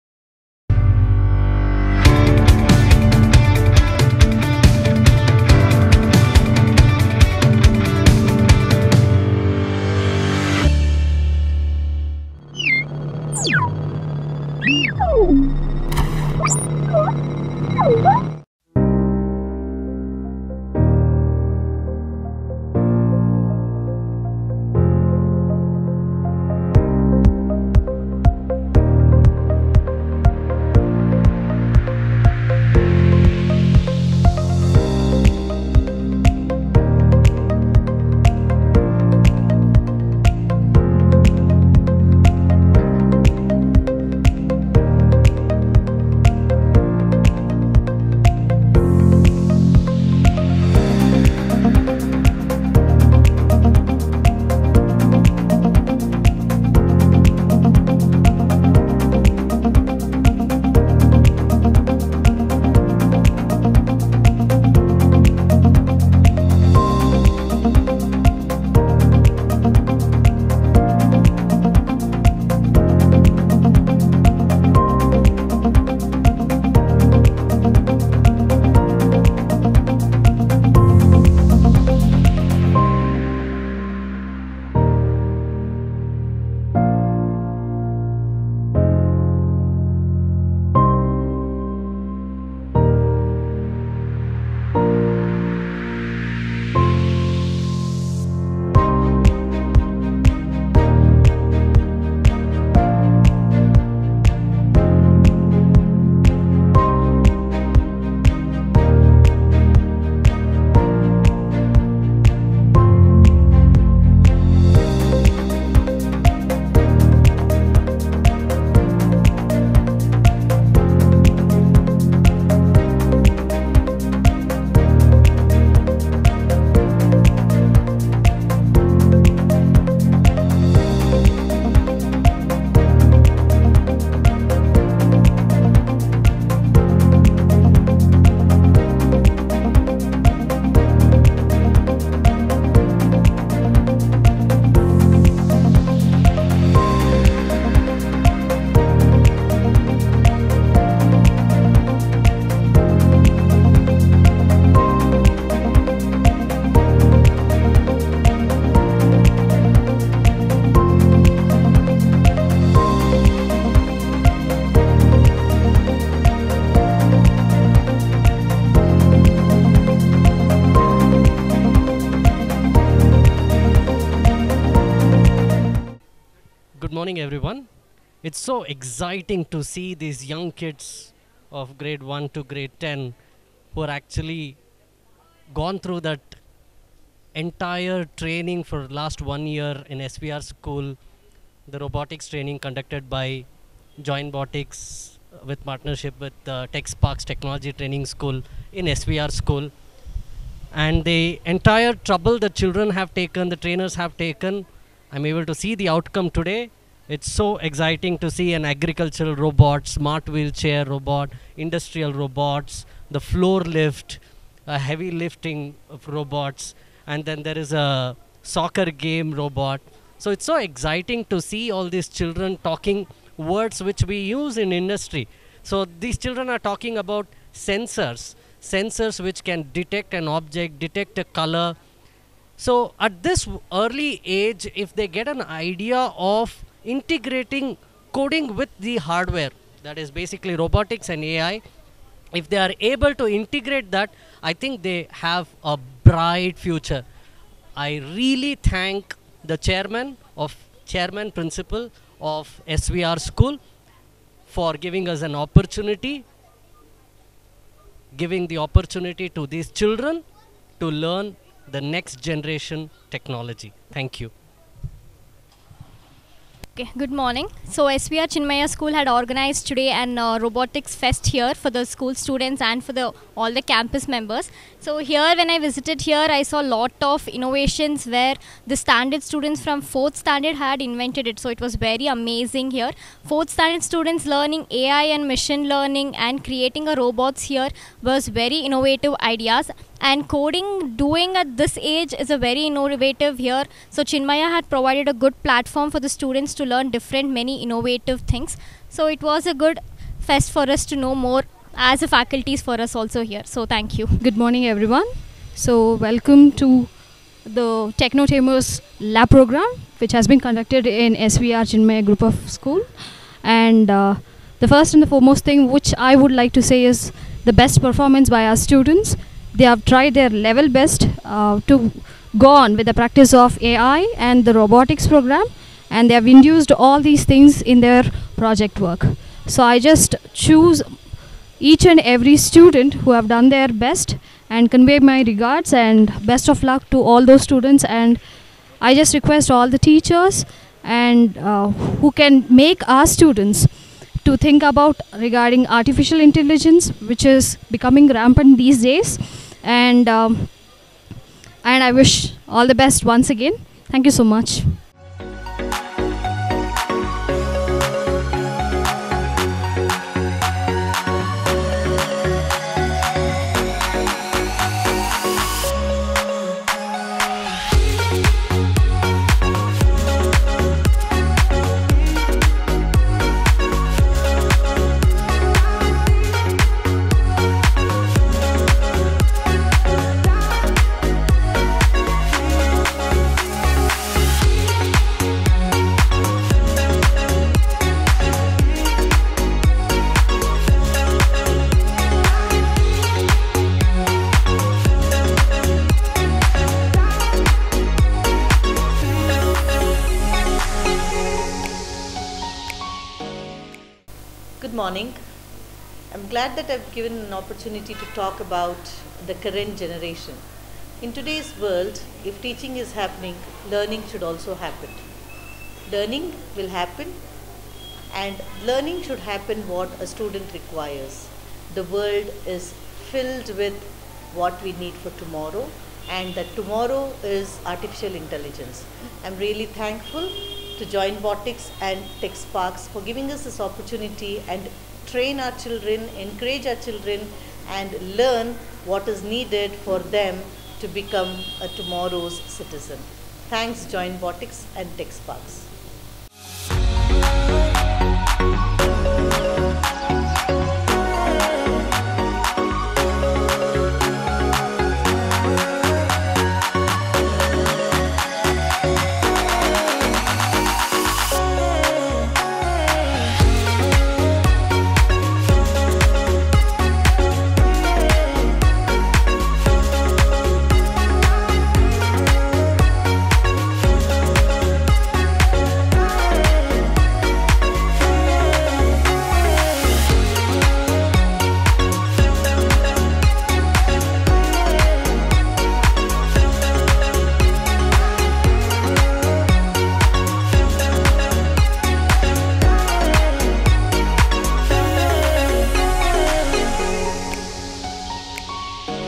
Robotics Carnival 2022 - 2023 | SVR Chinmaya School | TechSparx JoinBotics
Step back in time to the amazing Robotics Carnival at SVR Chinmaya School, hosted by TechSparx JoinBotics! 🤖🎪 Explore the cool stuff that was shown – robots made by clever students who love robots.

There was one special robot that helped farmers with their farming – it could do things like planting seeds and watering plants all by itself. Even though the carnival is done, you can still watch and remember how awesome it was. Hit the play button and see all the robots, especially the farming helper, again!